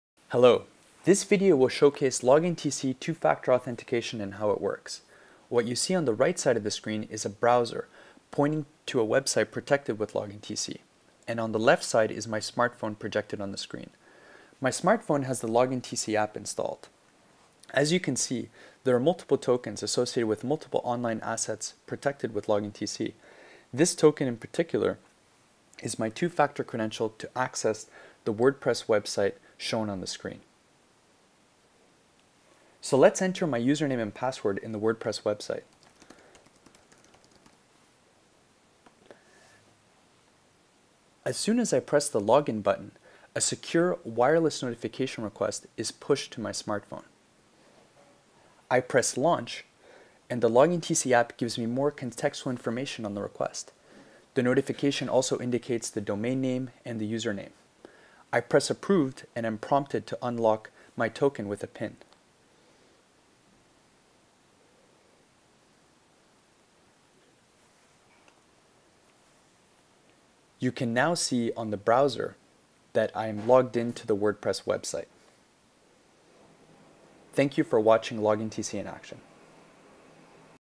LoginTC and WordPress
The video showcases LoginTC multi-factor authentication to access:

 - A WordPress website

The video is captured on a computer screen and a smartphone with the LoginTC app projected on the screen.